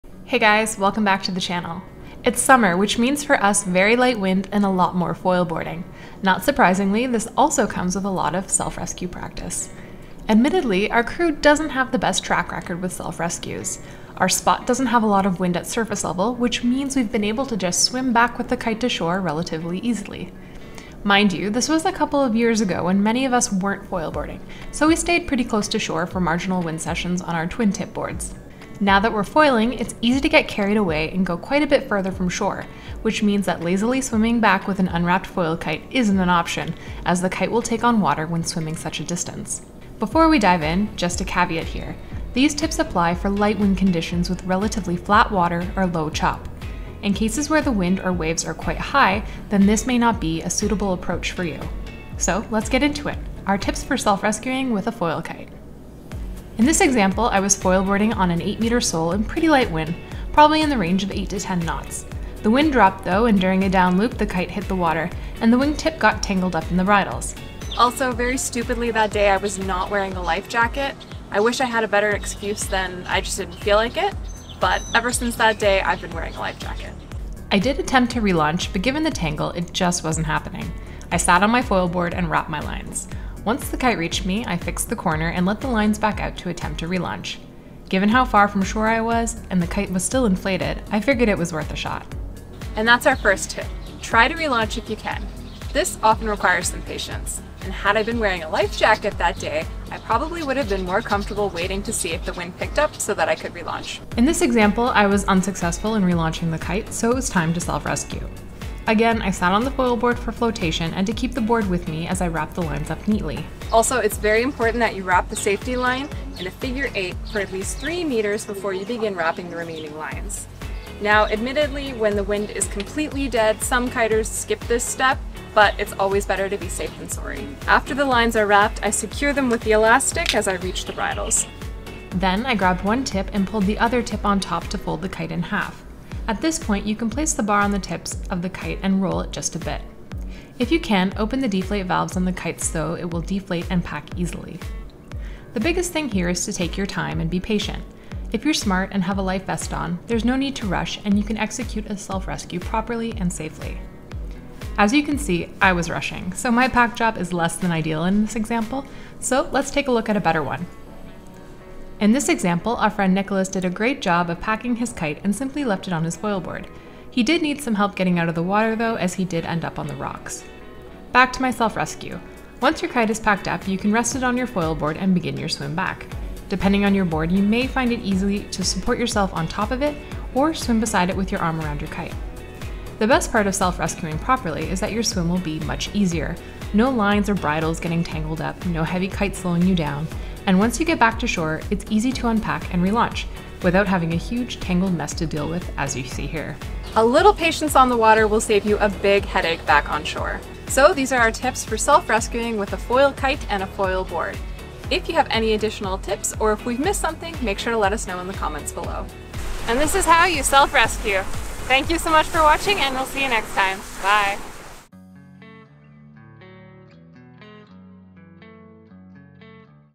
How to self rescue with a foil kite
In this video we show a real life example of a kitesurfing self rescue with a foil kite and foil board (hydrofoil board), as well as include our tips through the process. During this foilboarding session, the wind dropped and the kite went down, which led to a full self rescue including packing the kite and a long swim back to shore. Enjoy!